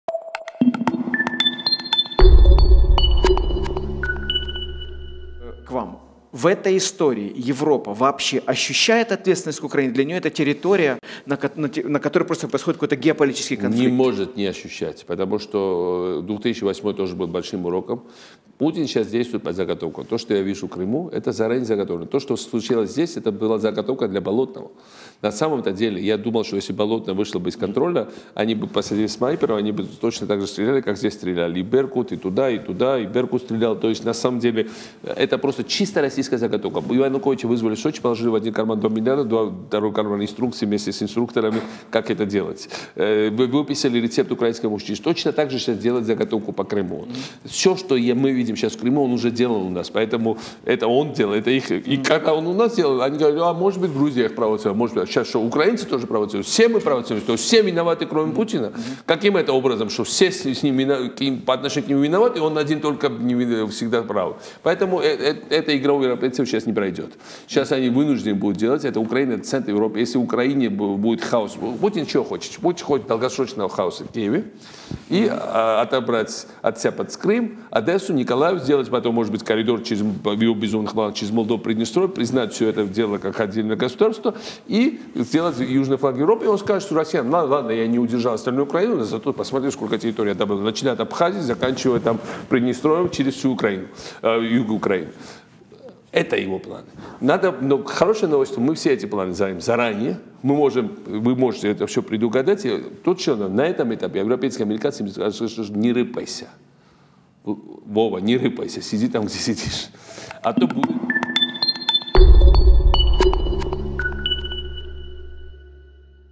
Міхеїл Саакашвілі: Події в Криму - заготовка Путіна
Президент Грузії (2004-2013) Міхеїл Саакашвілі на Громадському 26.02.14